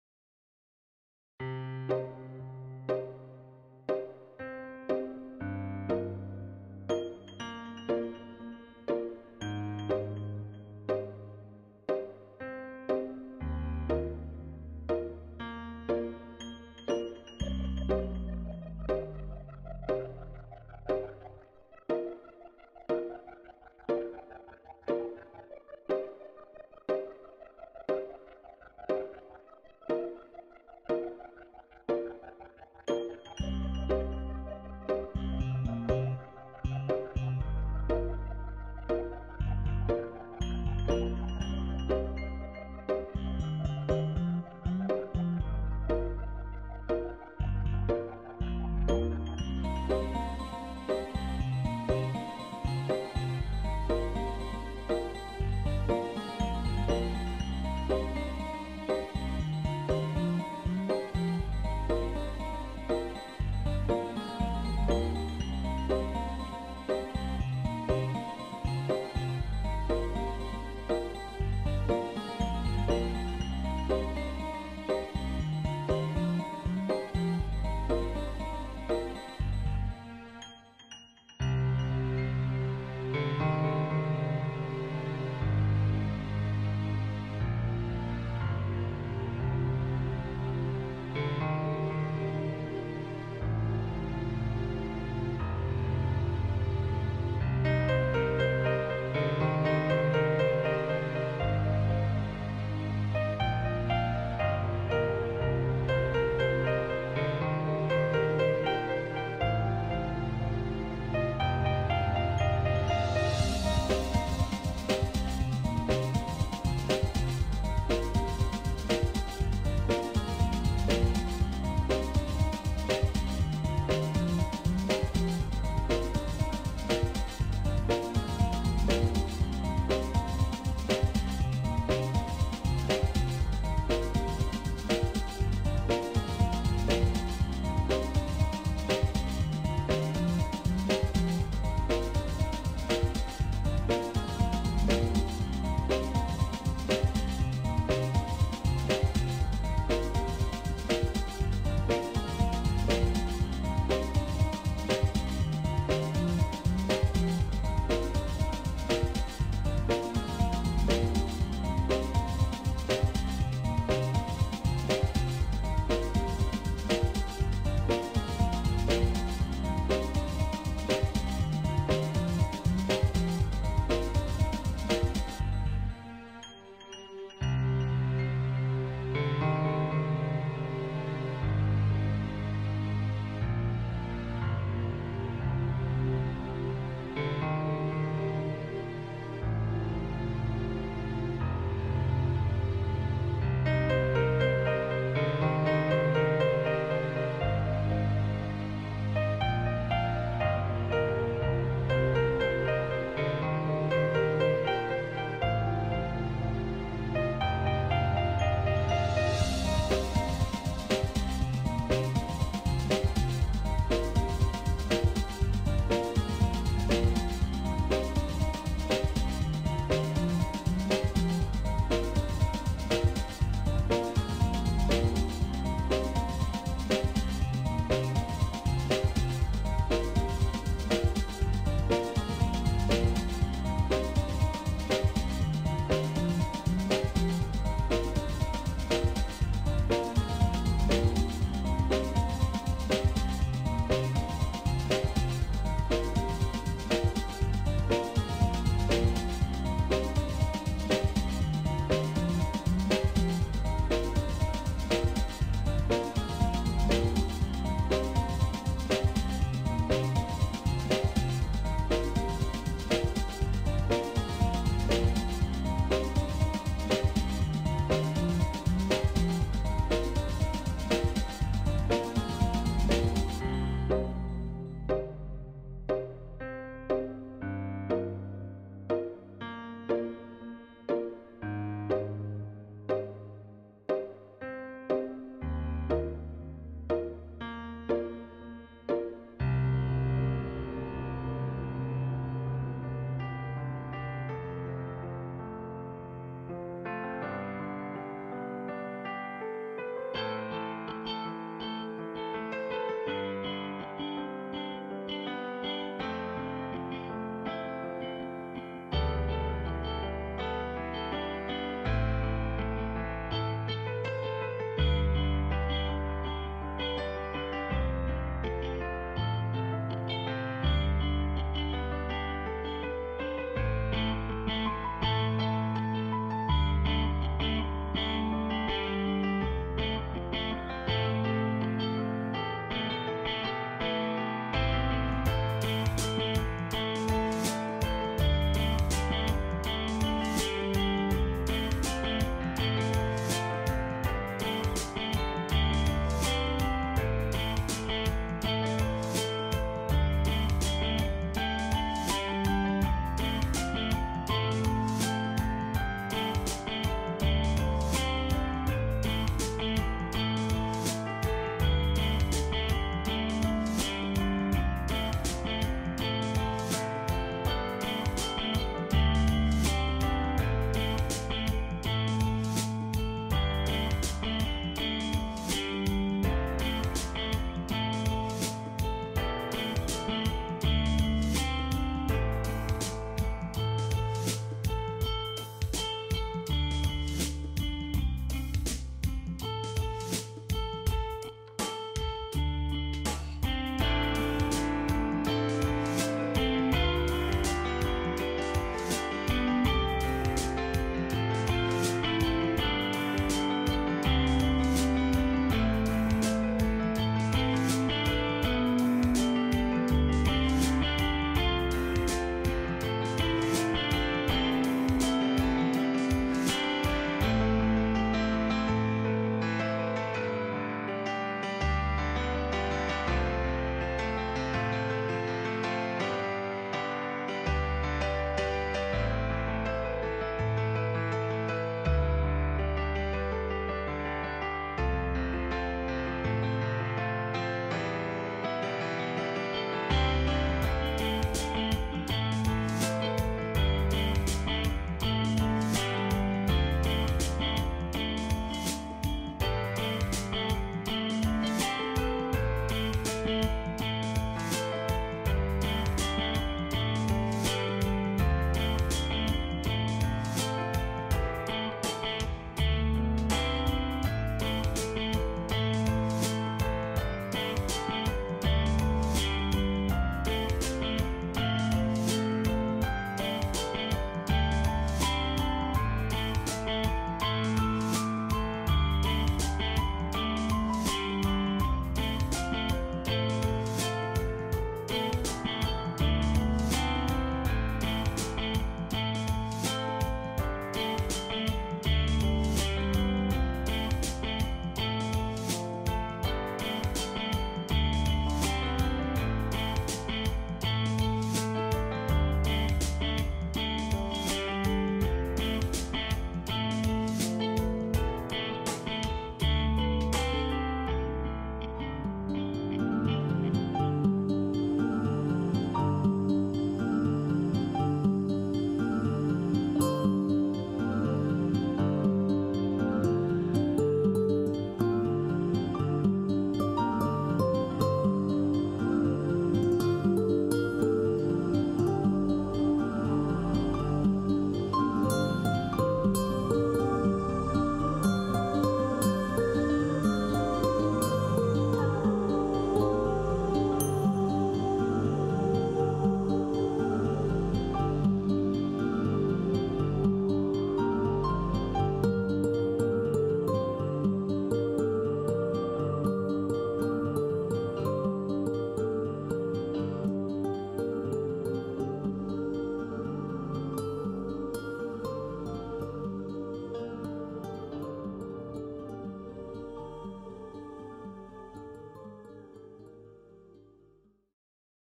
Timelapse from the North-East. Vol1
A few timelapse sequences from in and around the North-East of England.